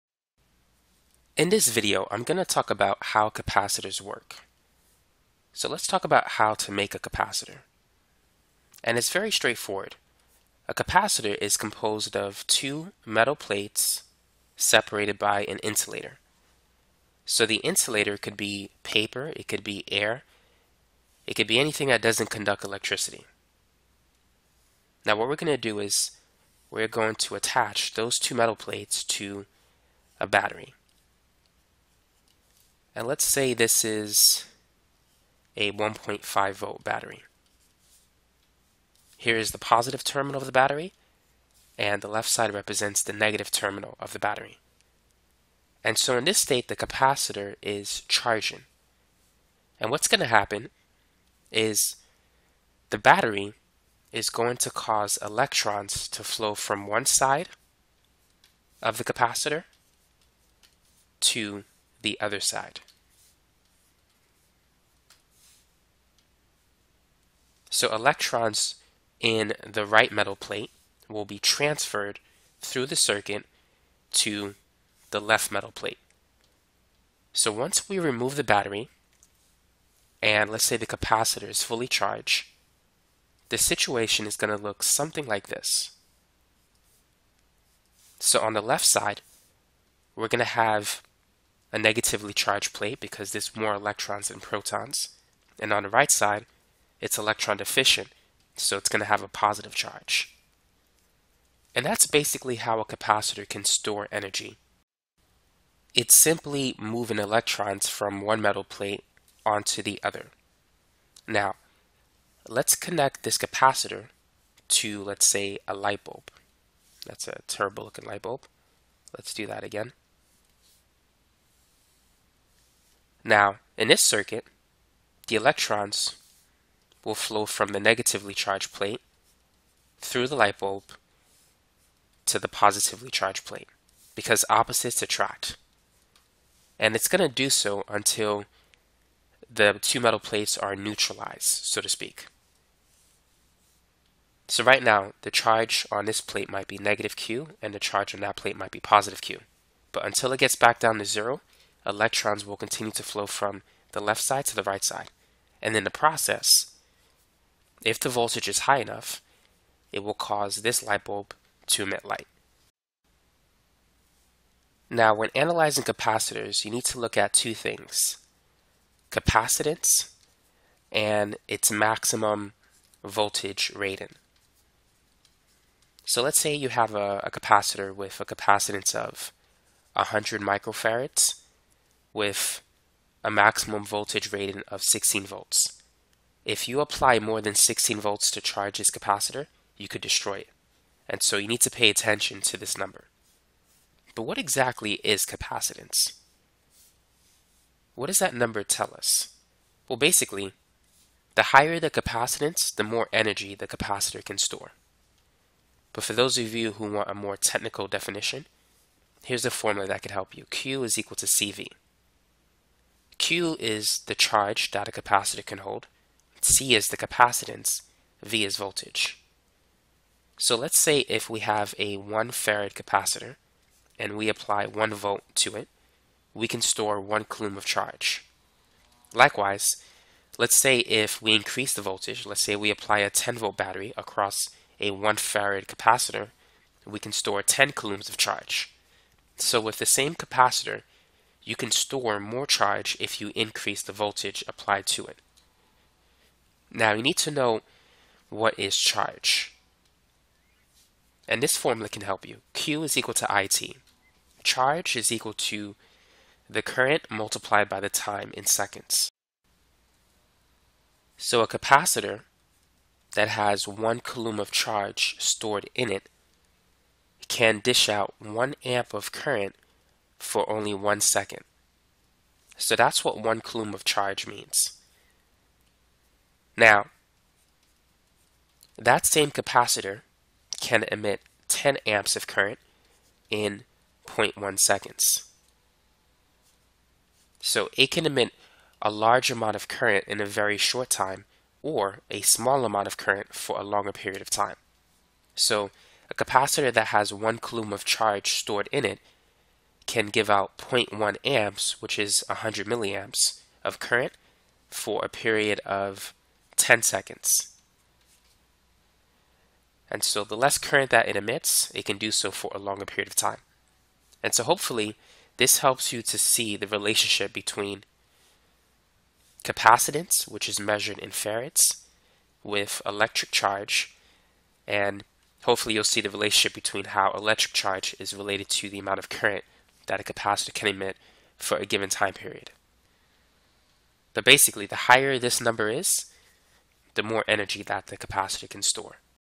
How Do Capacitors Work?
This electronics video tutorial explains how basic capacitors work when charging and discharging.  It covers topics such as capacitance, electric charge, and voltage.